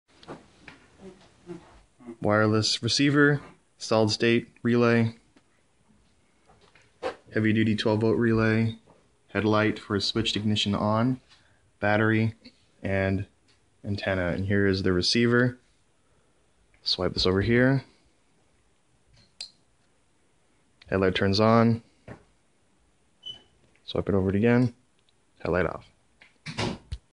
Motorcycle Keyless Ignition
Keyless ignition, the solid state relay and RFID receiver were about $30 total on Ebay.

I meant to say transmitter rather than receiver when showing the key fob